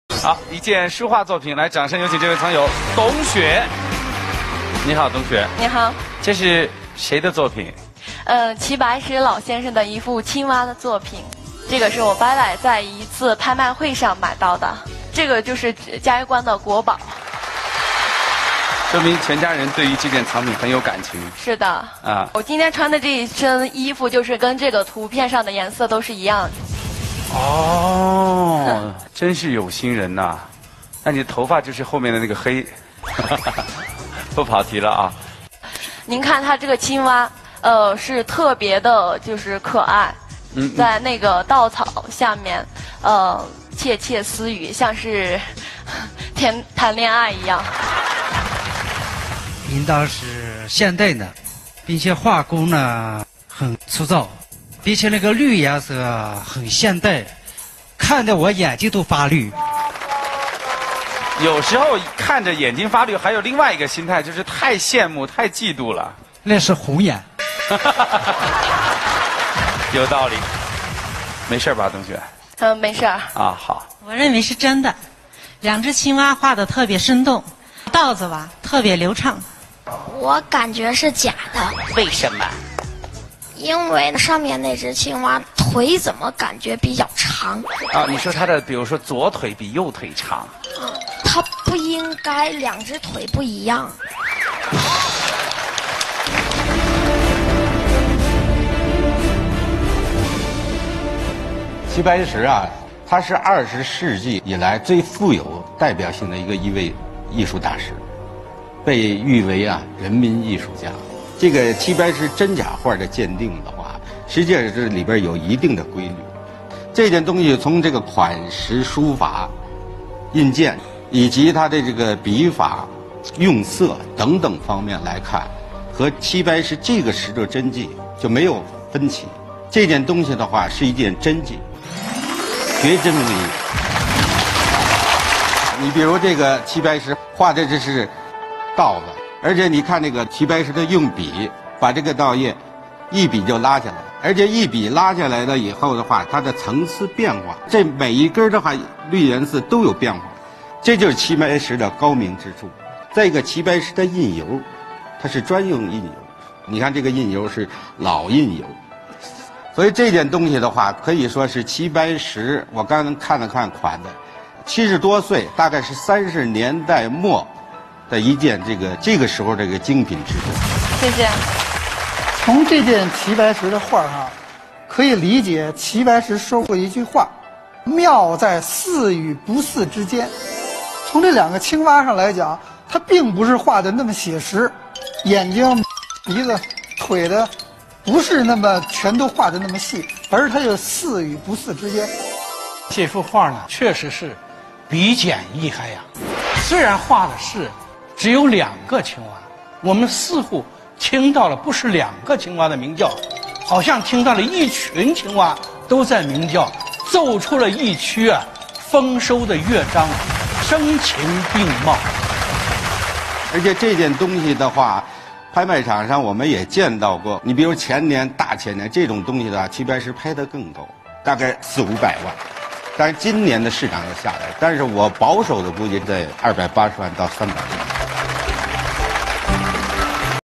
美女带齐白石的画鉴宝，专家竟估出天价，藏友：我眼睛都发绿！
❤ 每天都有新视频！！今天开始报名！！
👉每天更新最想的原创内容。
认证：高品质寻宝领域的创新者
# 小宝寻宝